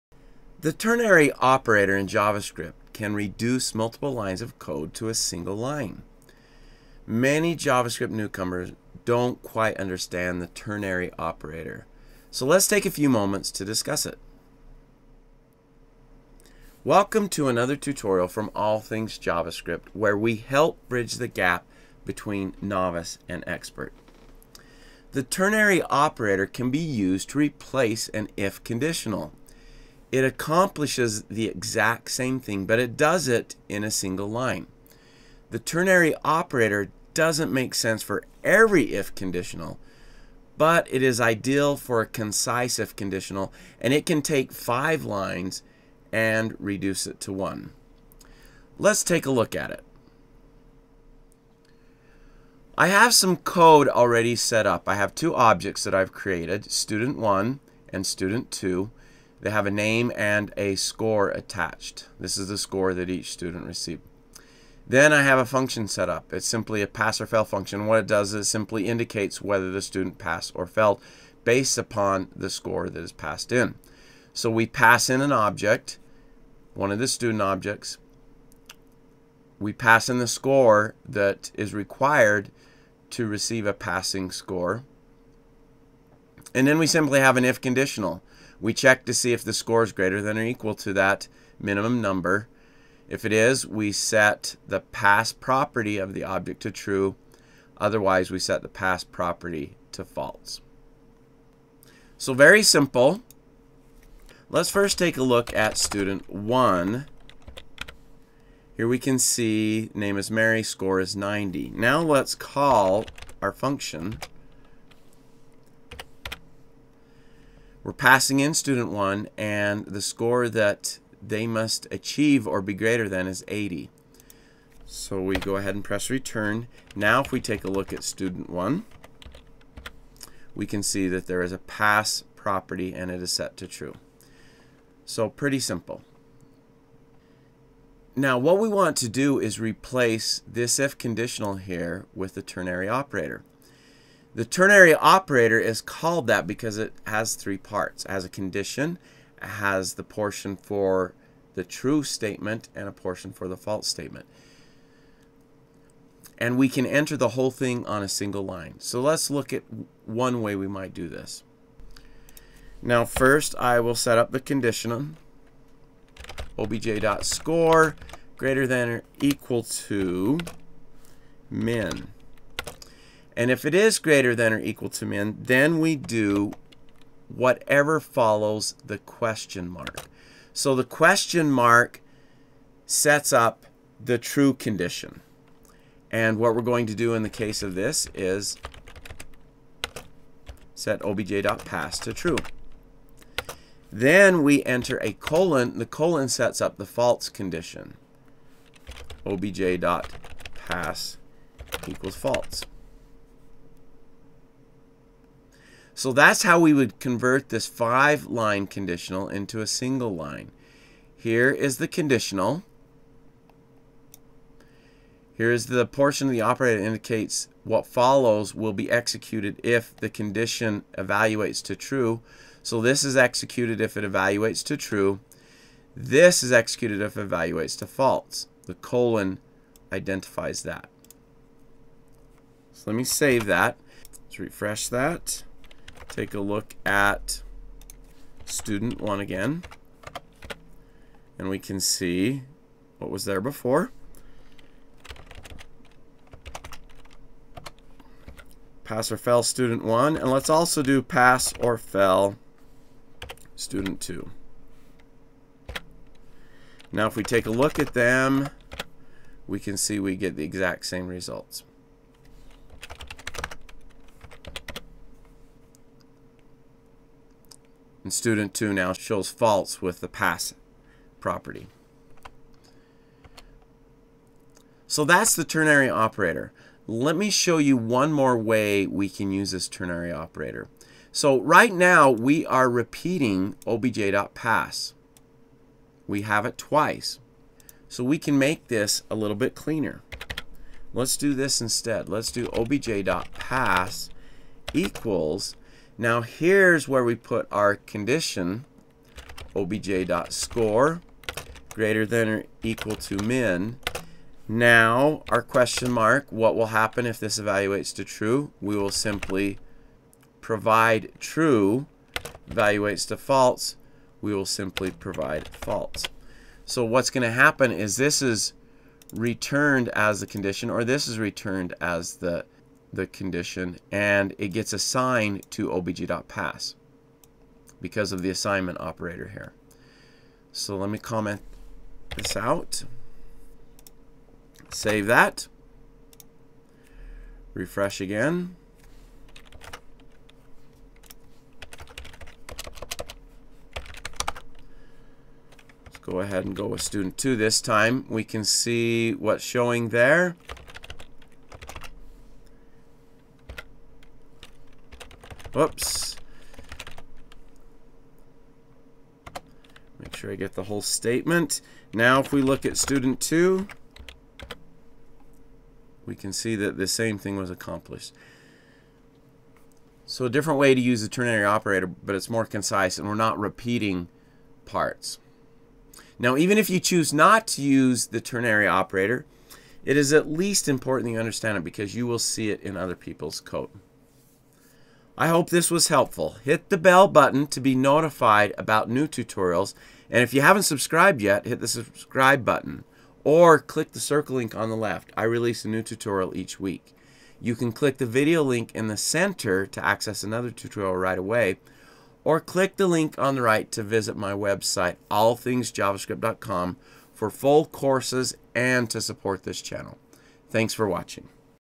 JavaScript Fundamentals: Understanding the Ternary Operator
The ternary operator allows you to reduce a conditional statement into a single line. Those just getting started with JavaScript sometimes do not understand the ternary operator. In this tutorial we go over it and provide examples.